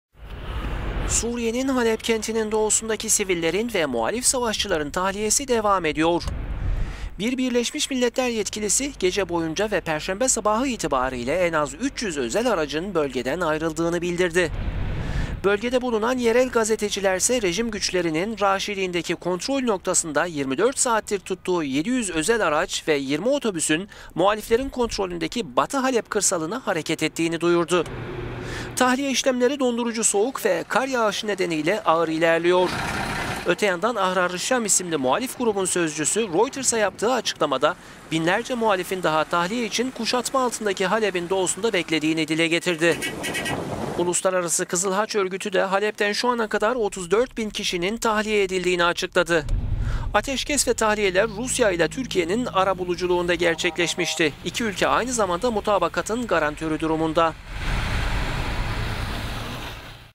Uluslararası Kızılhaç Örgütü: Halep'ten 34 bin kişi tahliye edildi
Suriye'nin Halep kentinin doğusundaki sivillerin ve muhalif savaşçıların tahliyesi devam ediyor.

Bir Birleşmiş Milletler yetkilisi, gece boyunca ve Perşembe sabahı itibarıyla en az üç yüz özel aracın bölgeden ayrıldığını bildirdi.

Bölgede bulunan gazeteciler, rejim güçlerinin Raşidin'deki kontrol noktasında 24 saattir tuttuğu 700 özel araç ve 20 otobüsün, muhaliflerin kontrolündeki batı Halep kırsalına hareket ettiğini duyurdu.

Tahliye işlemleri, dondurucu soğuk ve kar yağışı nedeniyle ağır…